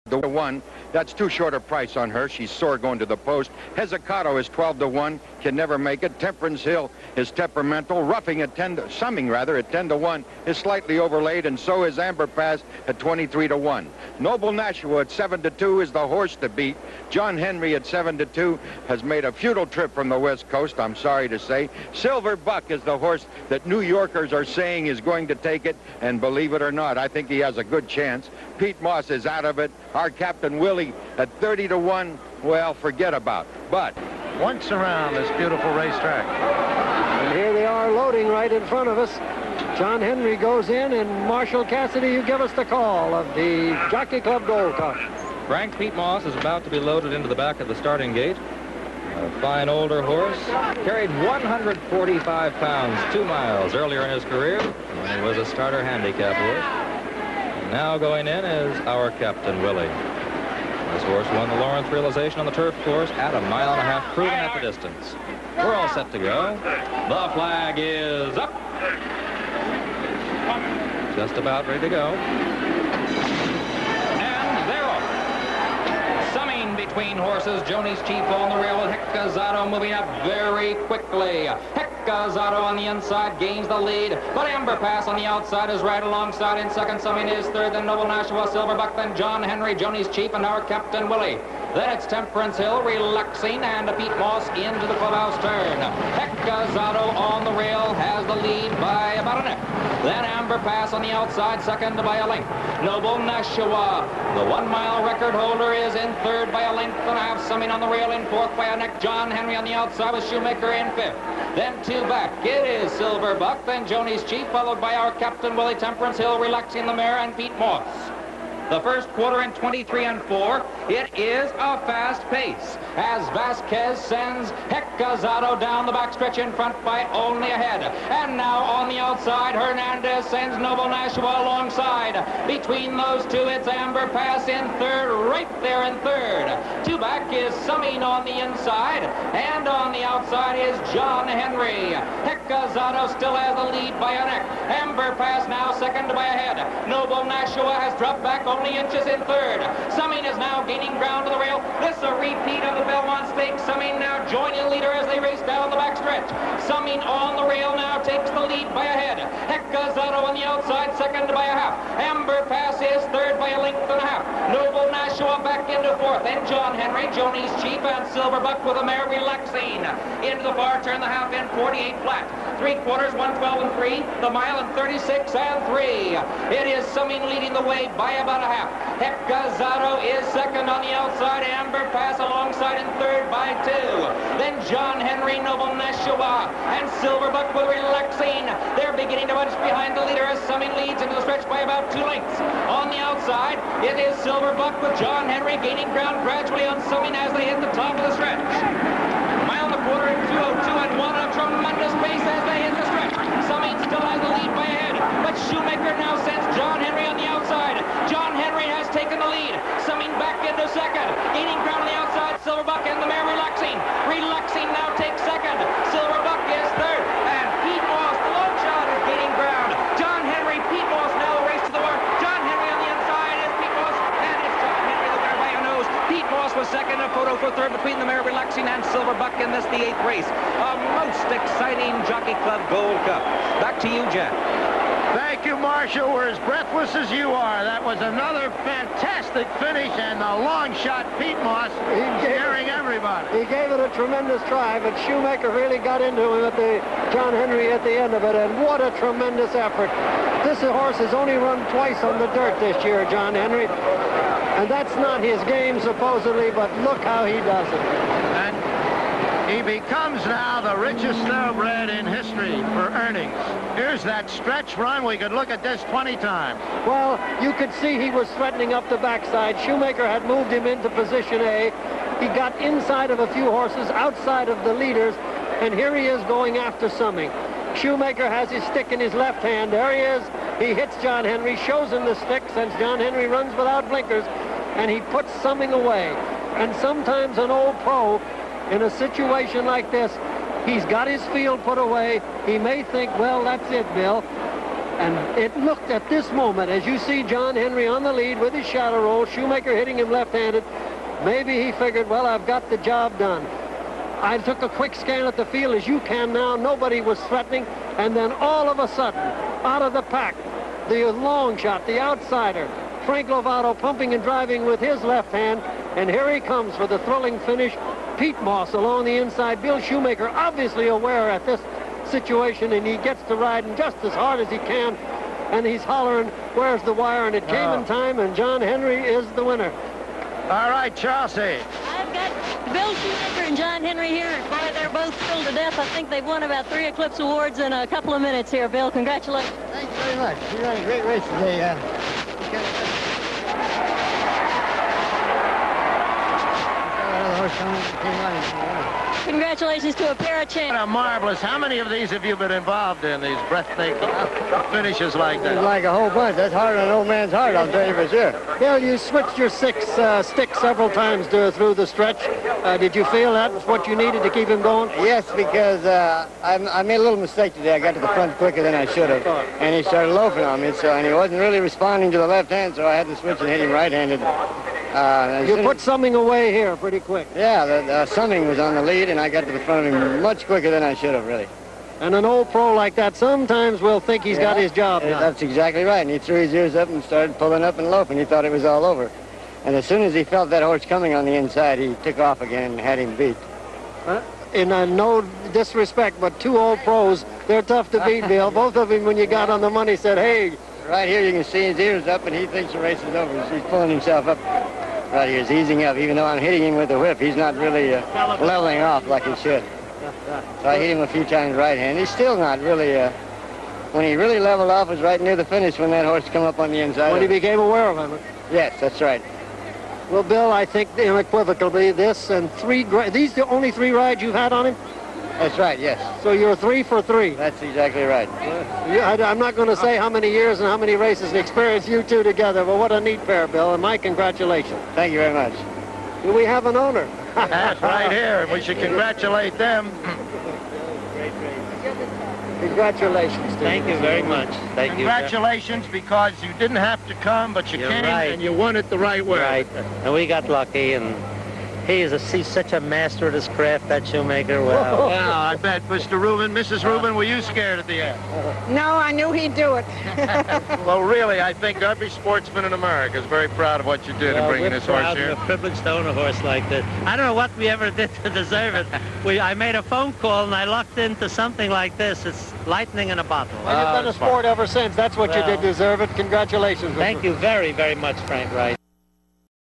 John Henry - 1981 Jockey Club Gold Cup
John Henry winning 1981 Jockey Club Gold Cup with post race comments